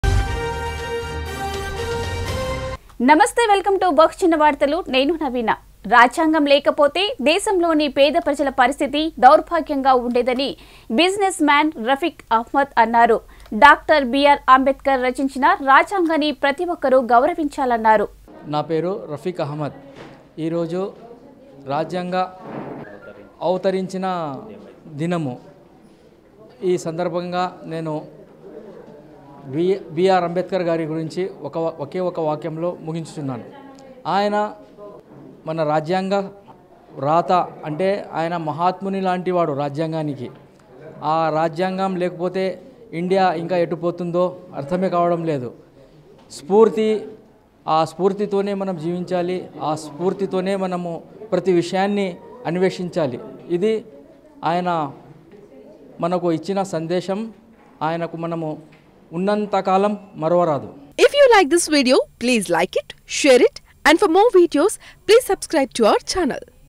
రాజ్యాంగం పవిత్రగ్రంధం గా భావించాలి....! | రఫిక్‌ అహ్మద్ | Ambedkar TV
రాజ్యాంగం పవిత్రగ్రంధం గా భావించాలి. ! | రఫిక్‌ అహ్మద్ |

రాజ్యంగం లేకపోతే దేశంలోని పేద ప్రజల పరిస్థితి దౌర్భగ్యంగా  ఉండేదని బిజినెస్‌ మ్యాన్‌  రఫిక్‌ అహ్మద్ అన్నారు.డాక్టర్‌  బిఆర్‌ అంబేడ్కర్‌ రచించిన  రాజ్యాంగాన్ని   ప్రతి ఒక్కరు గౌరవించాలన్నారు.